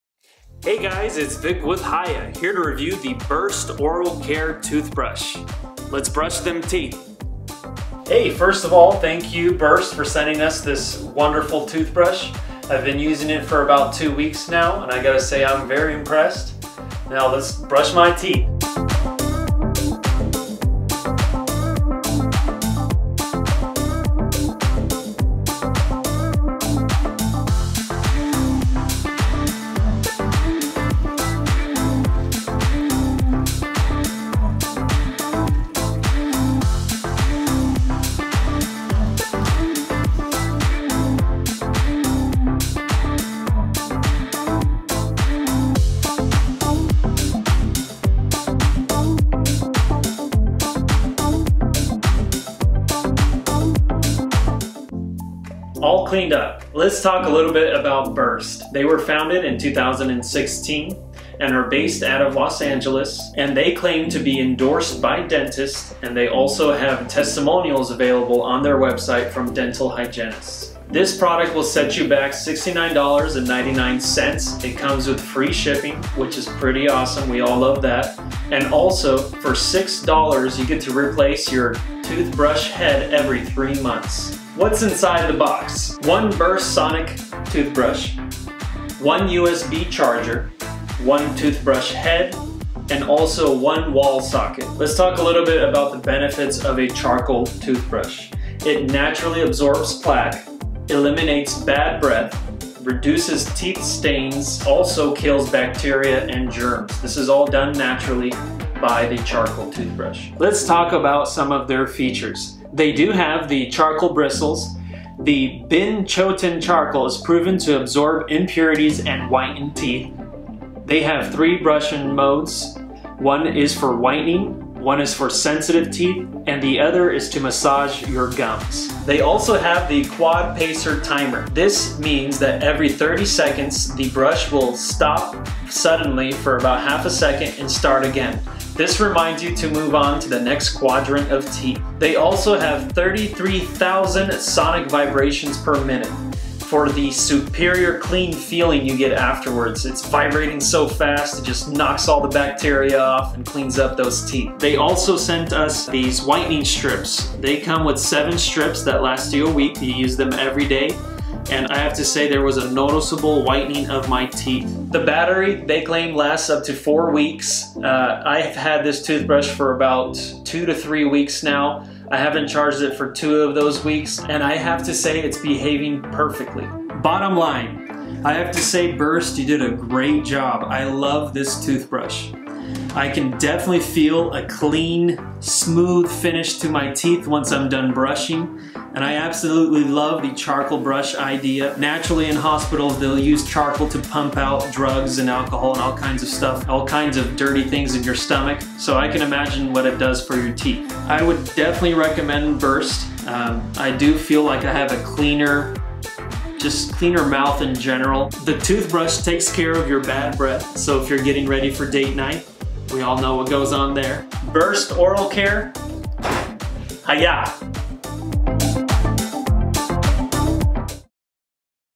The BURST Toothbrush Review
In this BURST Toothbrush review, based on two weeks worth of usage, we discuss the features, benefits and overall impressions of the toothbrush.